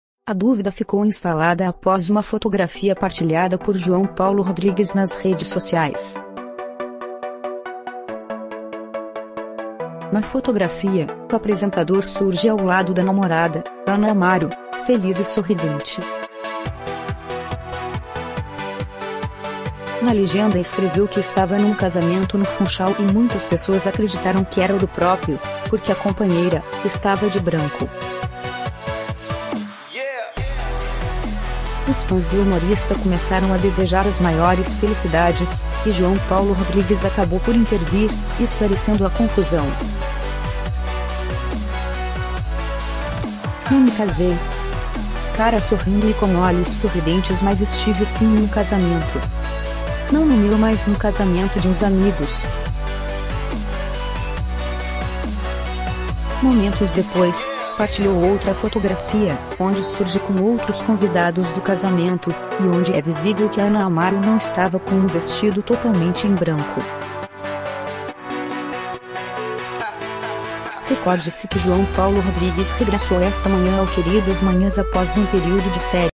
João Paulo Rodrigues casou-se em segredo? Apresentador já reagiu
Na fotografia, o apresentador surge ao lado da namorada, Ana Amaro, feliz e sorridente.
Na legenda escreveu que estava num casamento no Funchal e muitas pessoas acreditaram que era o do próprio, porque a companheira, estava de branco.
Os fãs do humorista começaram a desejar as maiores felicidades, e João Paulo Rodrigues acabou por intervir, esclarecendo a confusão. «Não me casei! 😊 Mas estive sim num casamento! Não no meu mas no casamento de uns amigos!».
Momentos depois, partilhou outra fotografia, onde surge com outros convidados do casamento, e onde é visível que Ana Amaro não estava com um vestido totalmente em branco.
Recorde-se que João Paulo Rodrigues regressou esta manhã ao Queridas Manhãs após um período de férias.